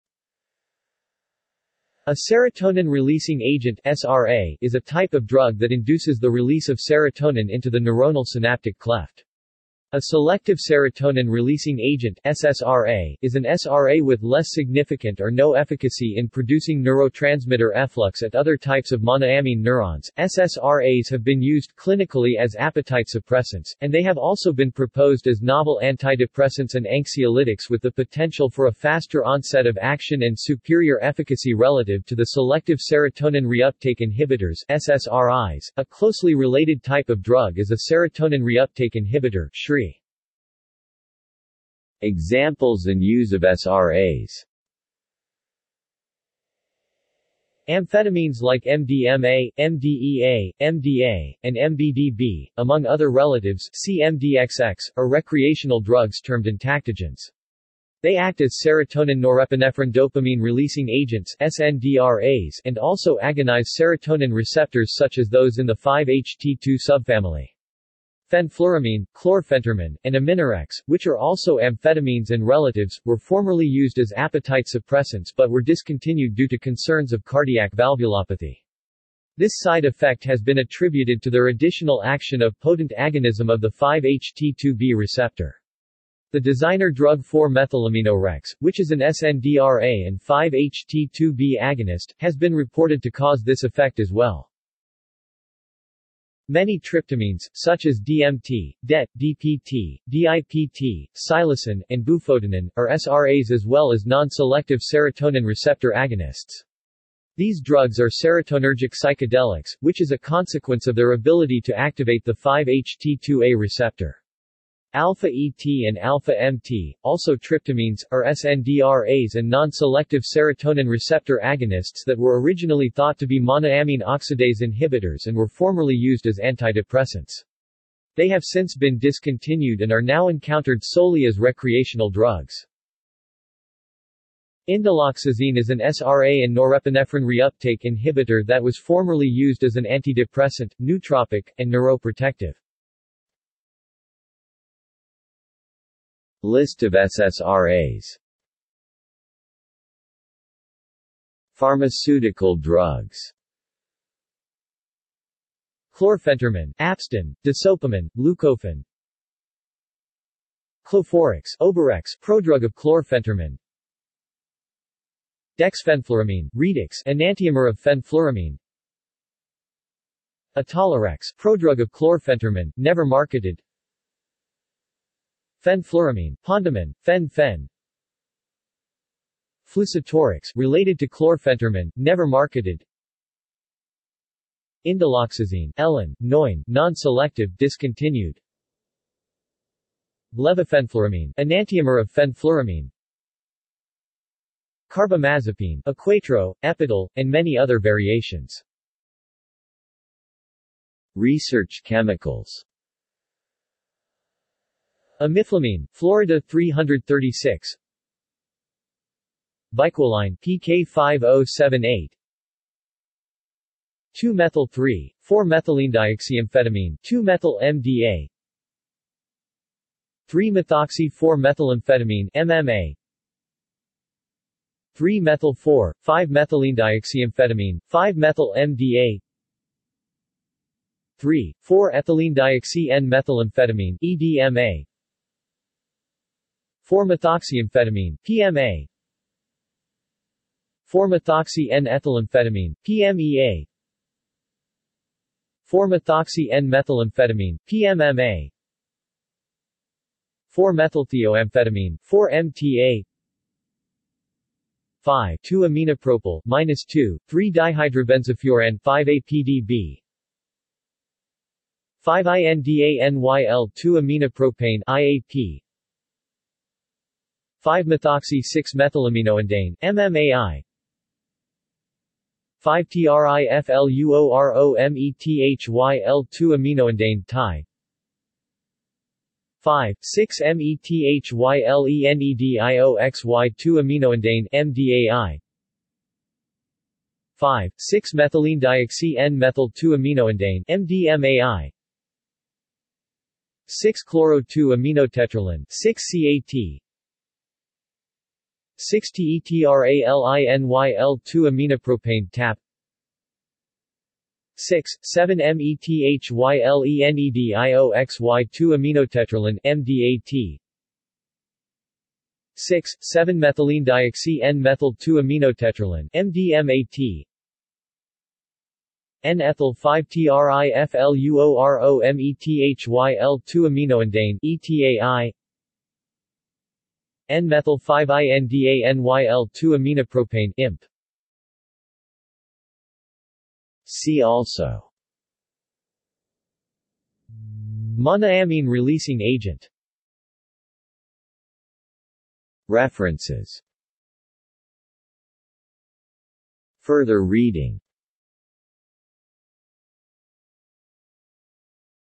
Serotonin releasing agents | Wikipedia audio article
This is an audio version of the Wikipedia Article:
Serotonin releasing agents

SUMMARY
A serotonin releasing agent (SRA) is a type of drug that induces the release of serotonin into the neuronal synaptic cleft. A selective serotonin releasing agent (SSRA) is an SRA with less significant or no efficacy in producing neurotransmitter efflux at other types of monoamine neurons.SSRAs have been used clinically as appetite suppressants, and they have also been proposed as novel antidepressants and anxiolytics with the potential for a faster onset of action and superior efficacy relative to the selective serotonin reuptake inhibitors (SSRIs).A closely related type of drug is a serotonin reuptake inhibitor (SRI).